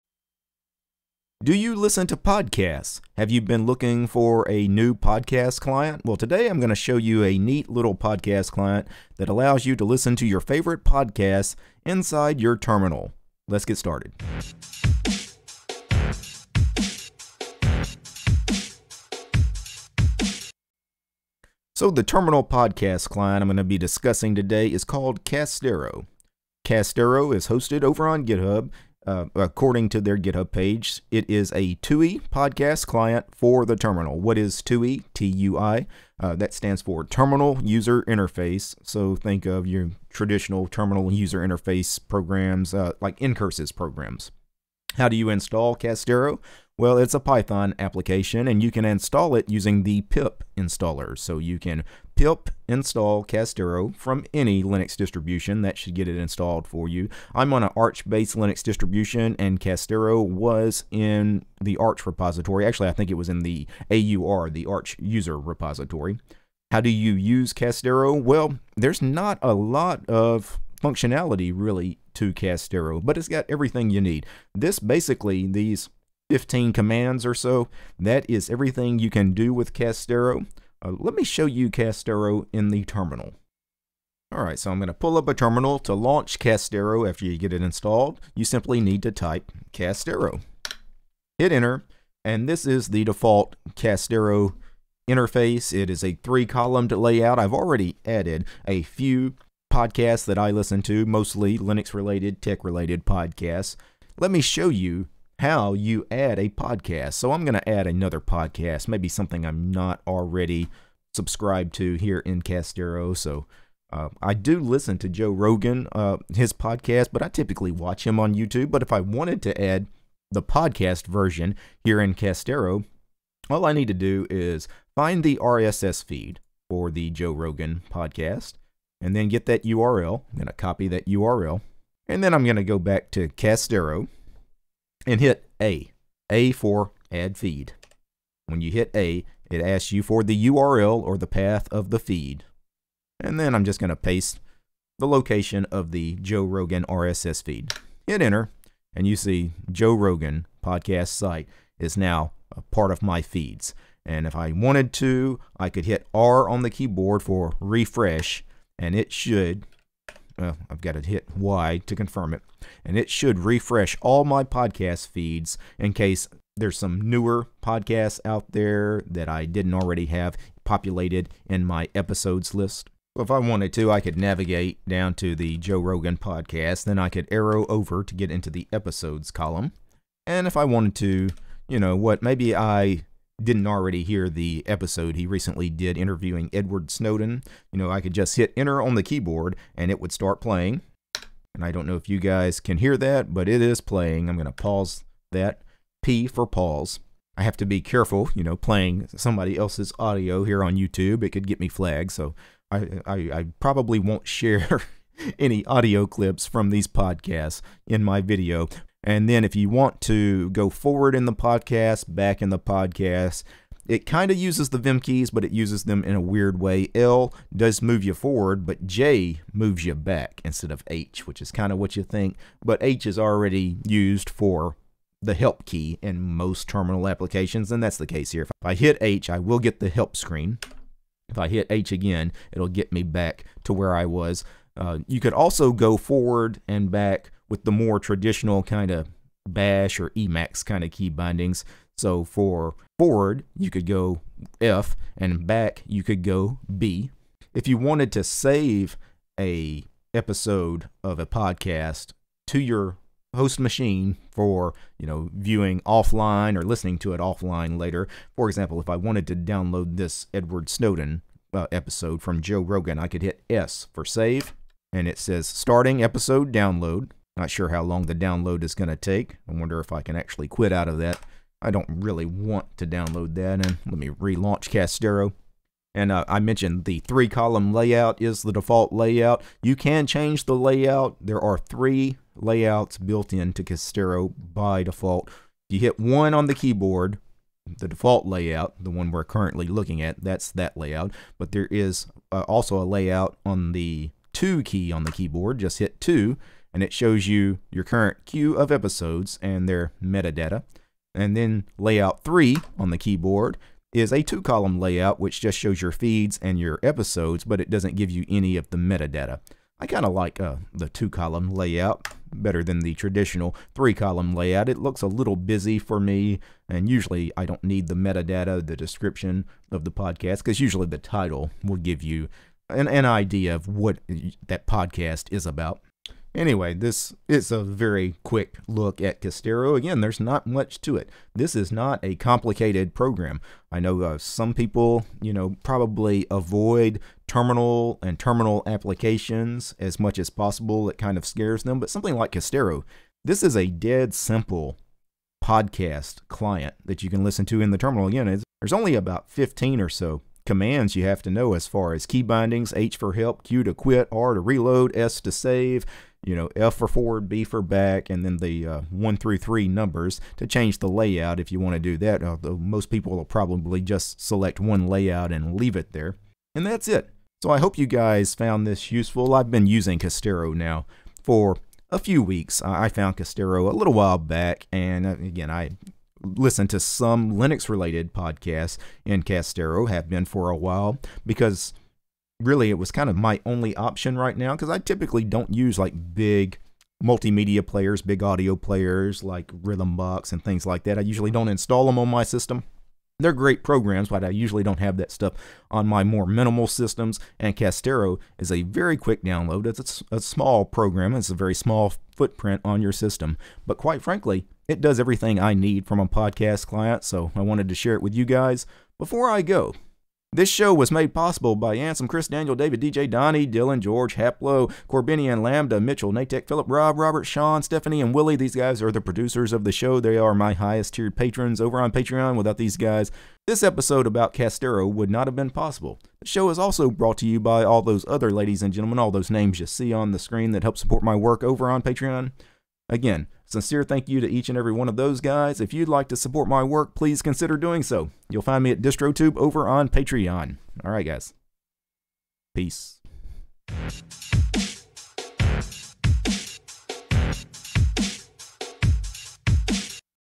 Listen To Podcasts In The Terminal With Castero
In this video, I'm going to discuss a neat terminal-based podcast client called Castero.  Light and minimal with just enough functionality built in to it.